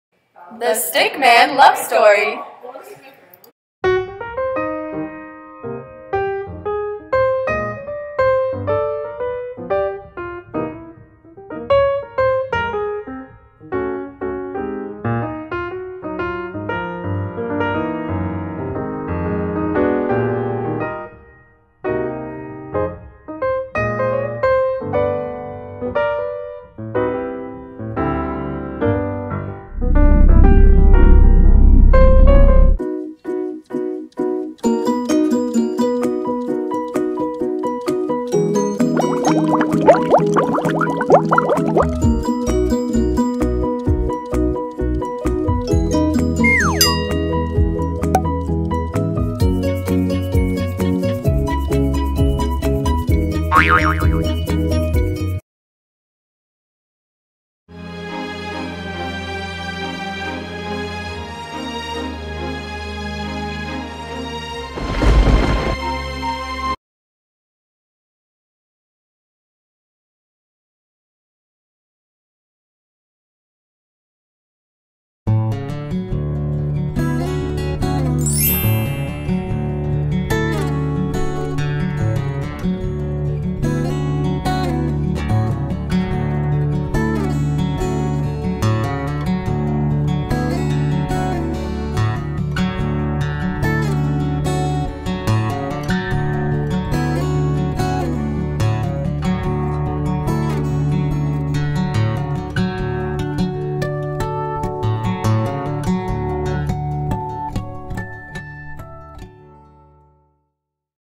Stickman Love Story (Stopmotion)
This is a project me and my friend did for our photography class :) We only had 3 days to make it and we took a little over 500 shots.